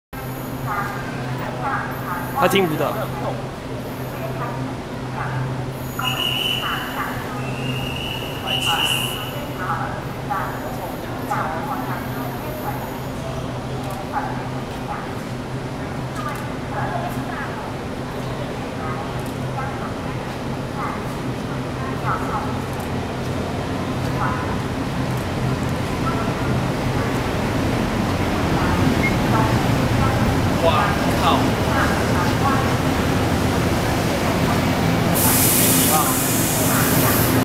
台鐵371次DR3100型自強號進站(屏東)
台鐵371次DR3100型自強號進站(屏東),攝於2015年8月23日,去屏東新站~